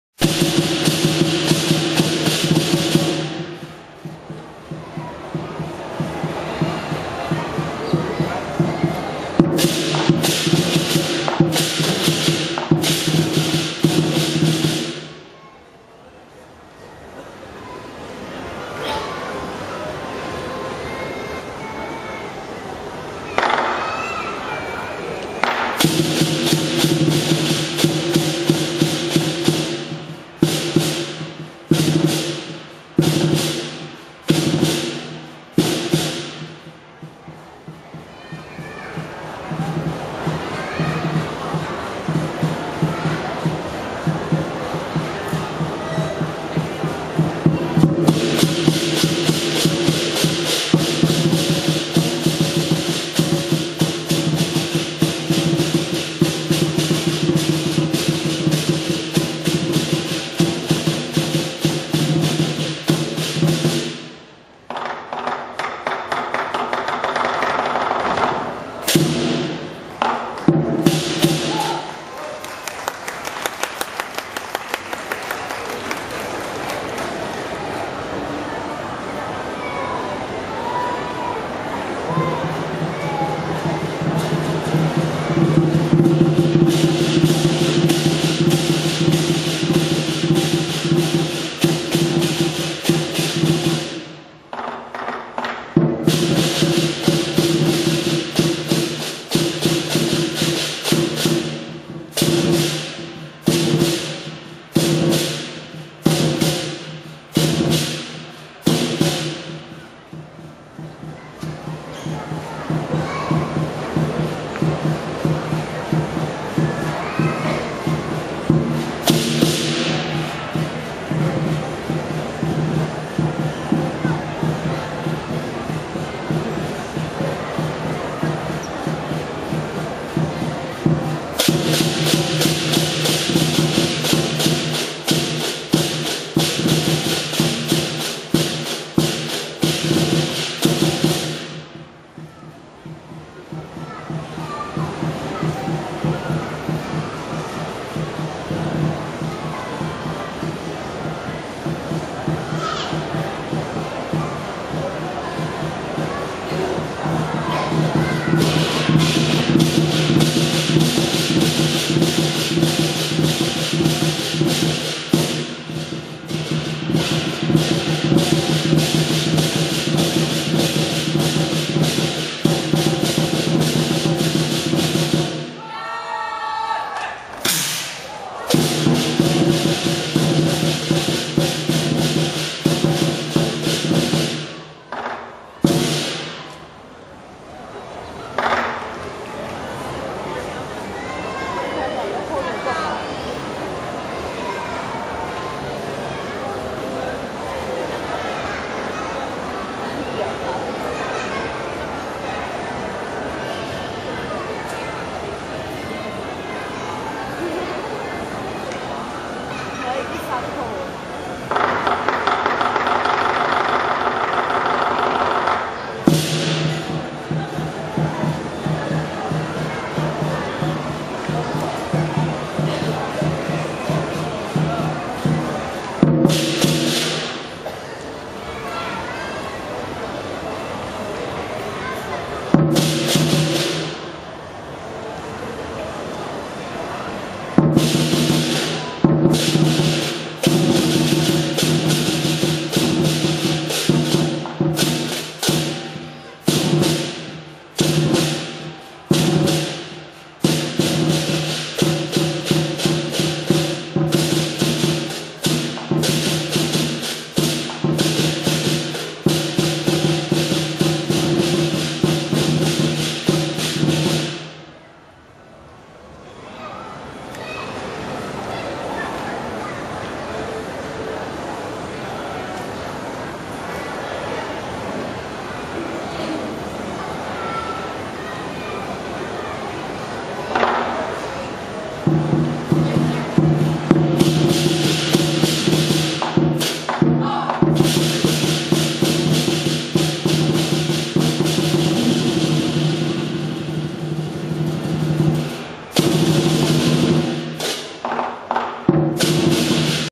Sutera Mall Chinese New Year High Pole Lion Dance
Chinese New Year High Pole Lion Dance @ Sutera Mall, Sutera Utama, Johor Bahru.